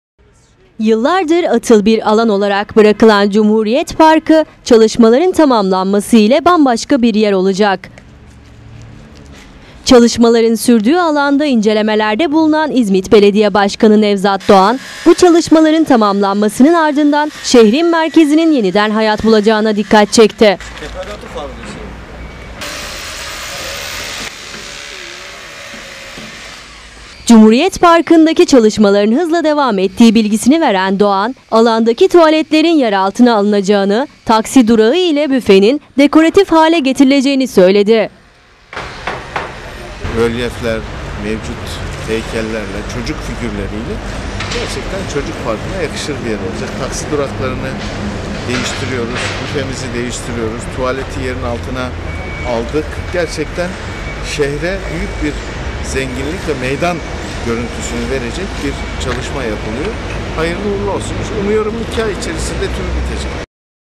KOCAELİ TV - CUMHURİYET PARKI'NDA SONA DOĞRU
Şehir estetiği ve modern şehirleşme kapsamında önemli projelere imza atan İzmit Belediye Başkanı Nevzat Doğan, Cumhuriyet Parkı'nda yapılan çalışmaları yakından takip ediyor. Çalışmaların sürdüğü alanda incelemelerde bulunan Doğan çalışmalar hakkında da detaylı bilgi aldı.